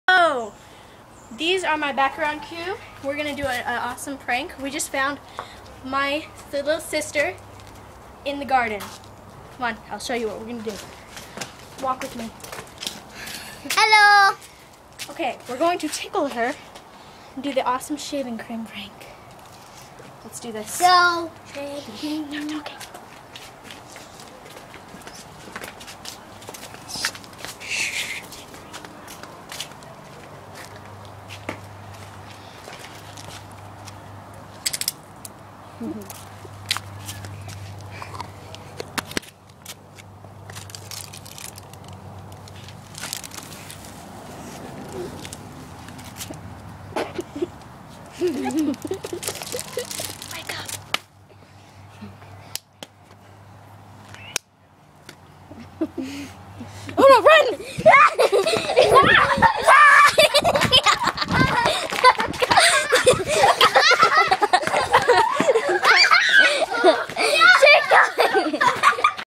Shaving cream prank
Shaving cream on sleeping person